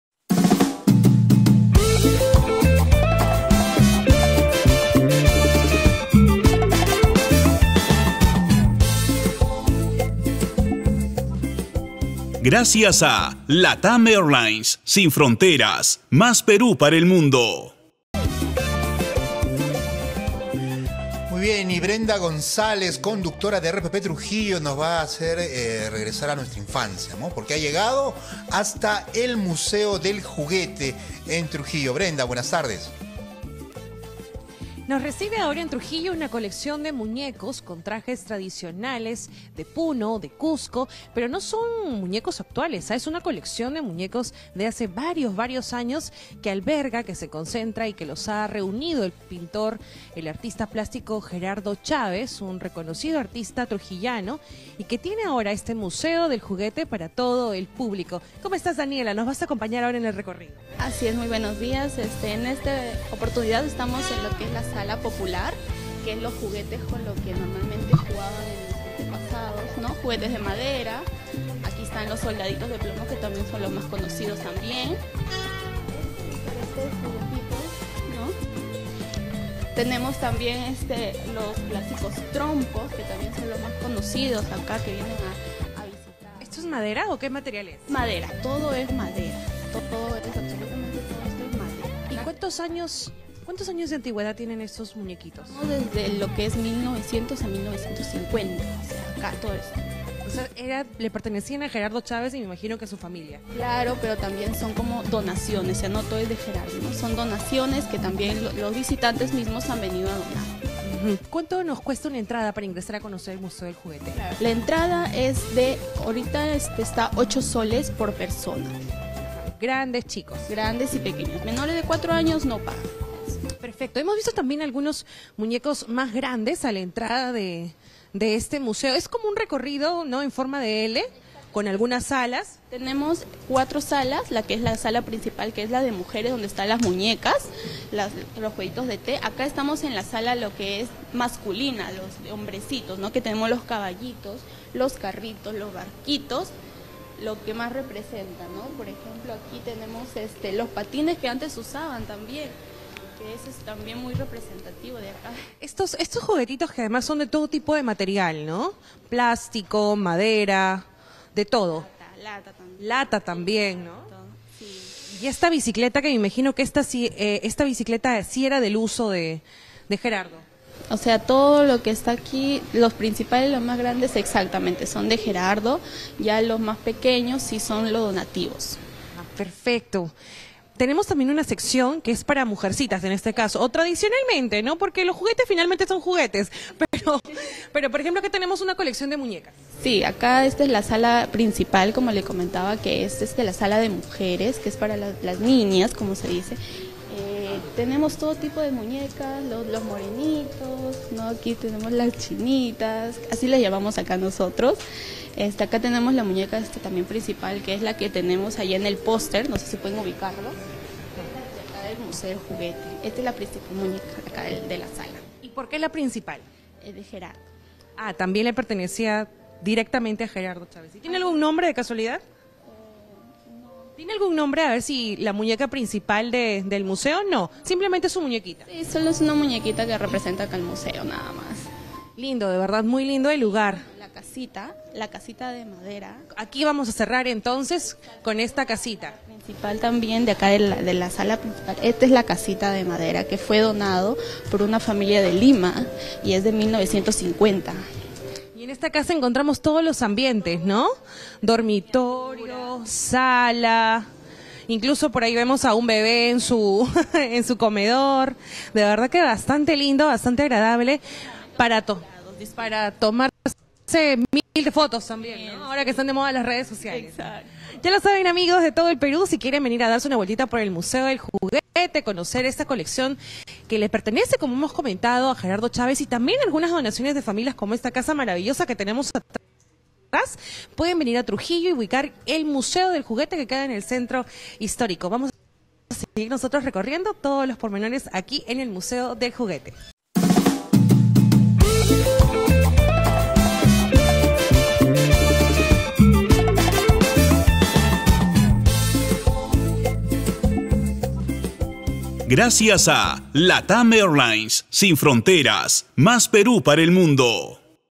Vistamos el Museo del Juguete en Trujillo
Visitamos el Museo del Juguete, en el Centro Histórico de Trujillo, una colección de muñecos reunidos por el artista plástico trujillano Gerardo Chávez López.